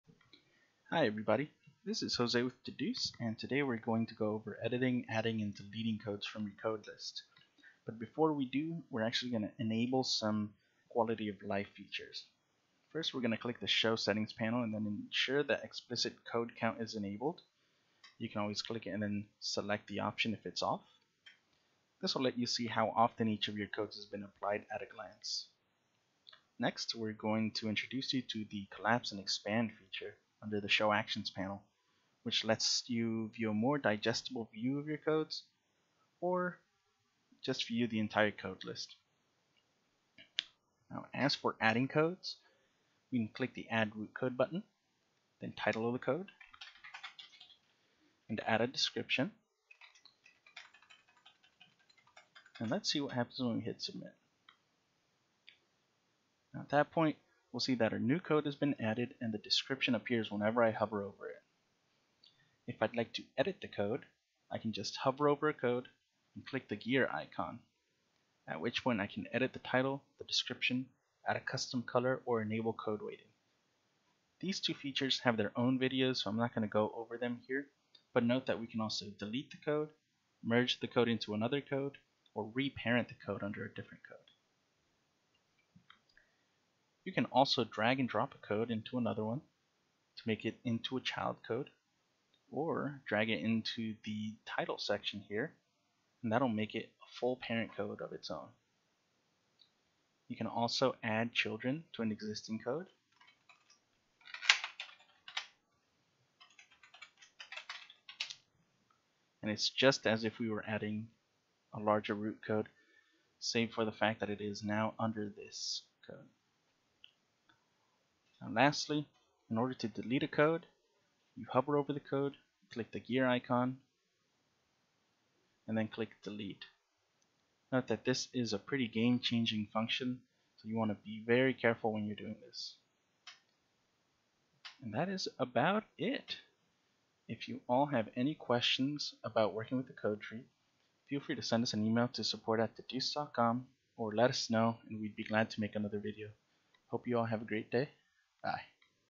Adding, Editing, Deleting Codes | Qualitative and Mixed Methods Analysis | Great Research Made Easy
Learn how to set up and maintain your code list, as well as use some nice quality of life features with our latest tutorial video!